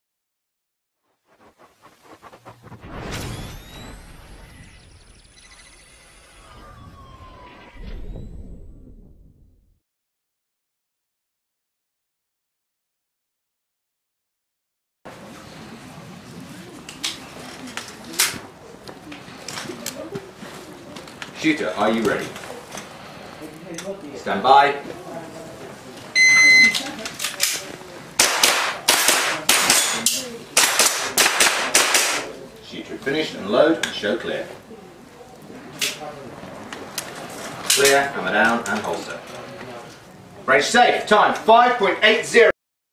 Airsoft Surgeon European Championship 2013 Video 47
FPS shooter from the Isle of Wight in action during the second day of the Airsoft Surgeon European Championship held last 19-20 October 2013 at The Grange in Balsall Common, Coventry, United Kingdom.